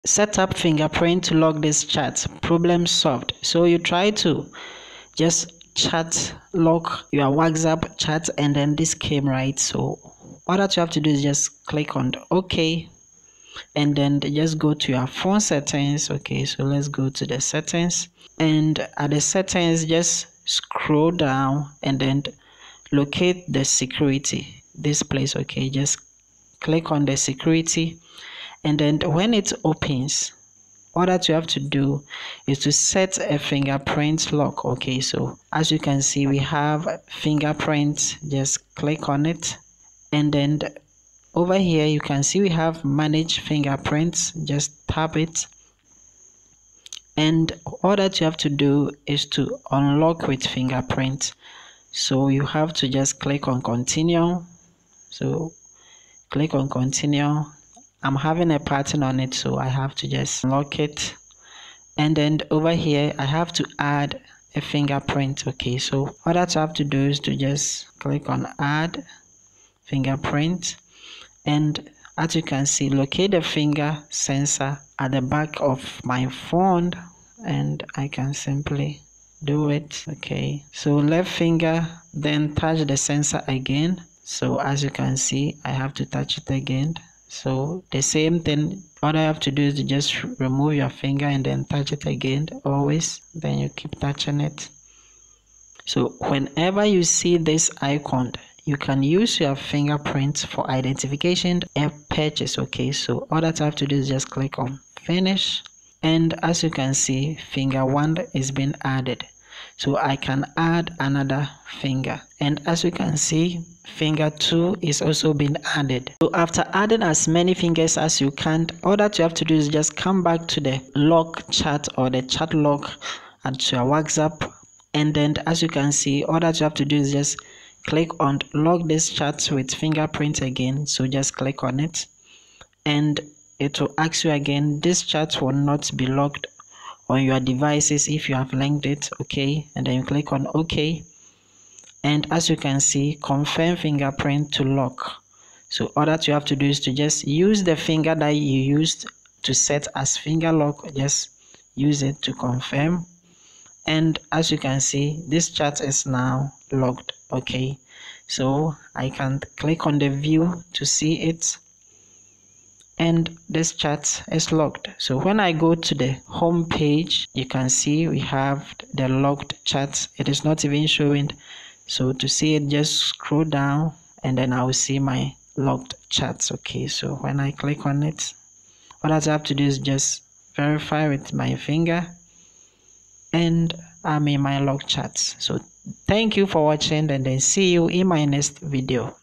Set up fingerprint to lock this chat (Problem Solved)
Set up fingerprint to lock this chat a step by step guide to solve this problem.

Whatsapp now gives you the chance to lock your chat and make it more private and before you do it you will need to use your finger to lock it and it becomes a challenge if you don't know your way out.

In this video you will learn how to set up fingerprint to lock this chat following a step by step guide.